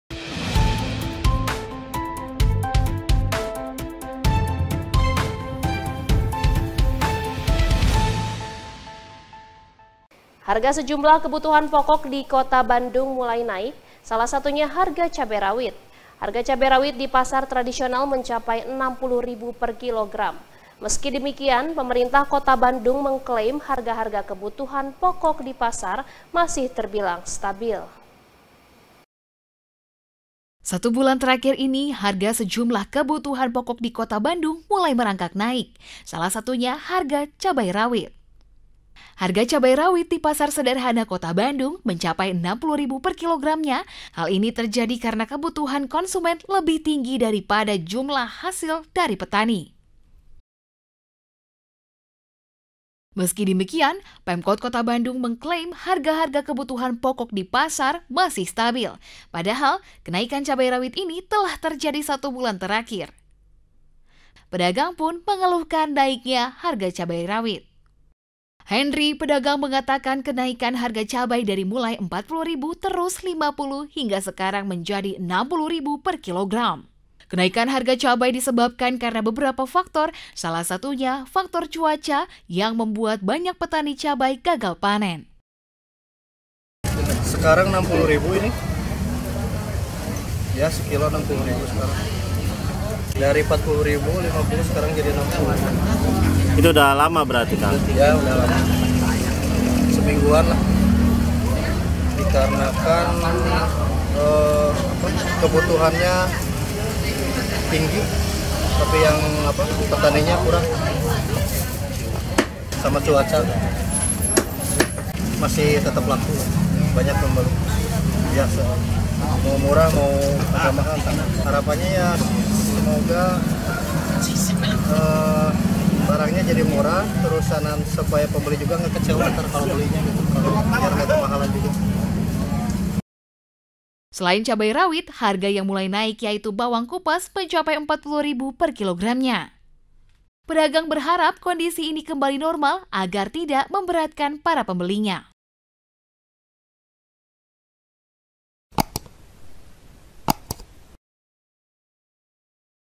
Harga Cabai Rawit di Kota Bandung Tembus 60 Ribu Per Kilo | UN News | UTV
Harga sejumlah kebutuhan pokok di Kota Bandung mulai naik, salah satunya harga cabai rawit. Harga cabai rawit di pasar tradisional mencapai 60 ribu per kilogram. Meski demikian, pemerintah Kota Bandung mengklaim harga-harga kebutuhan pokok di pasar masih terbilang stabil